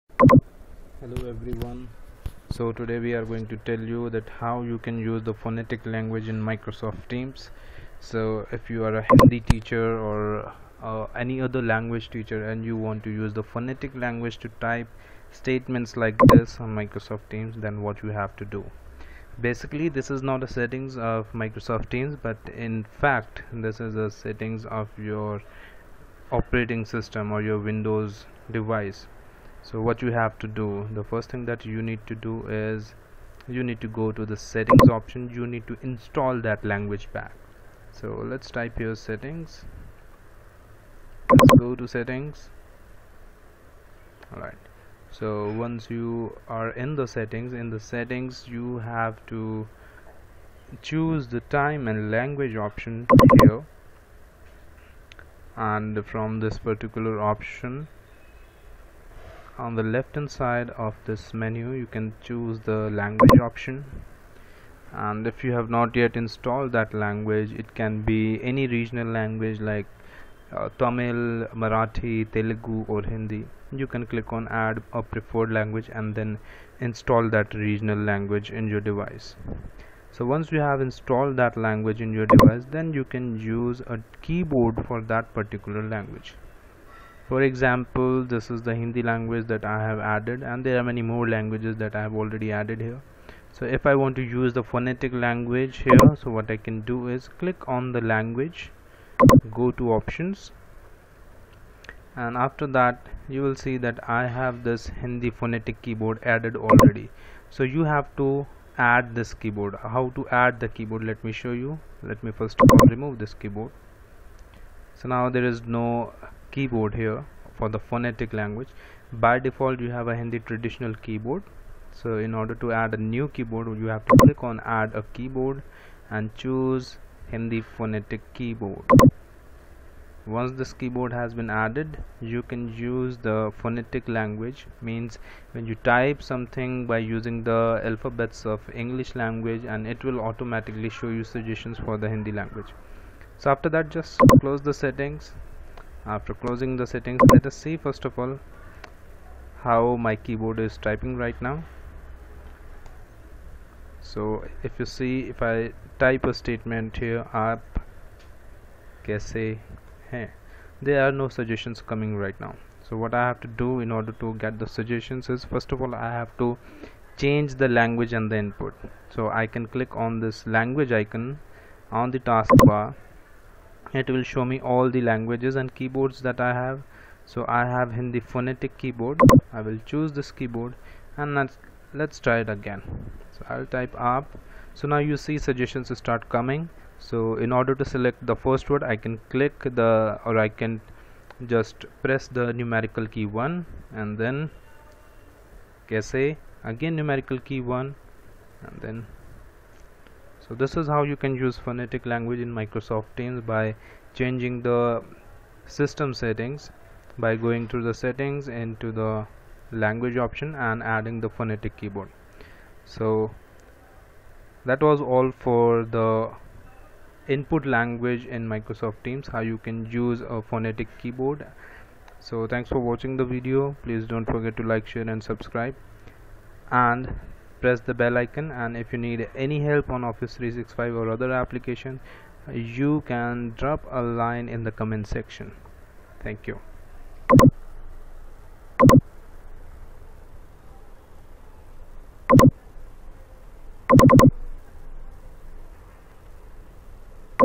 Hindi Typing in Microsoft Teams : Phonetic Typing SOLVED!!!
Use phonetic language to type messages in teams.